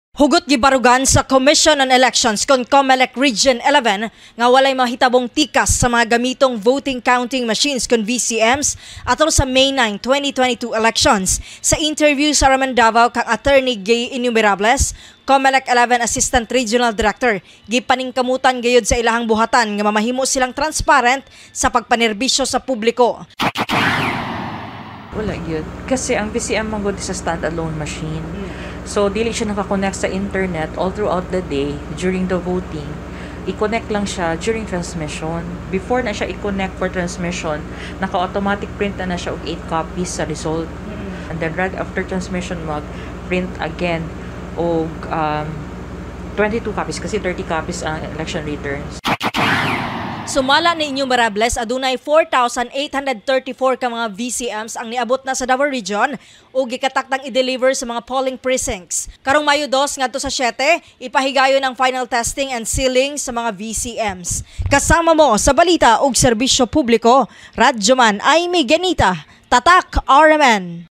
COMELEC XI: WALAY TIKAS SA VCM, TRANSPARENCY GIPANIGURO
DXDCNews: Hugot gibarugan sa Commission on Elections (COMELEC) Region XI nga walay tikas sa mga gamitong Vote Counting Machines sa May 9, 2022 elections.

Atangi ug paminawa ang mga balita sa DXDC RMN Davao 621 kHz AM radio dial.